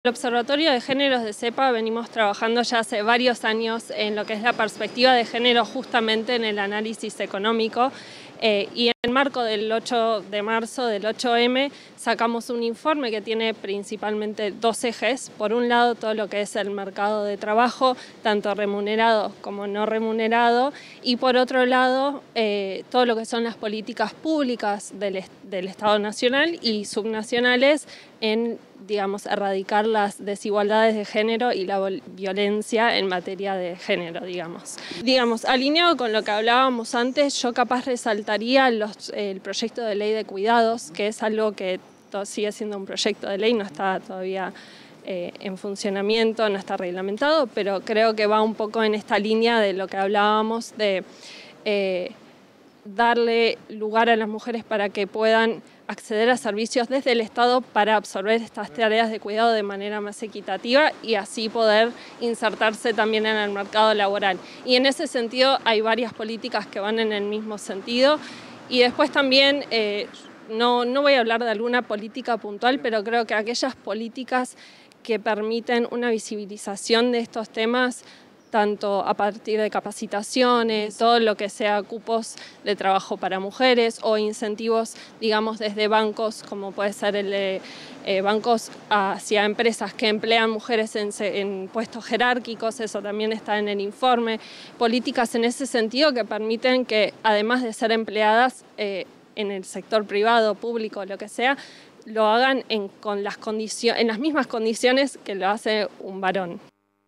Las brechas de género en la economía argentina
El 2022 mostró índices de recuperación económica, producción y empleo, aunque persistieron las desigualdades de género, según un informe del Centro de Economía Política Argentina (CEPA). Las mujeres ganaron menos, trabajaron más y son la mayoría de las personas en empleos informales.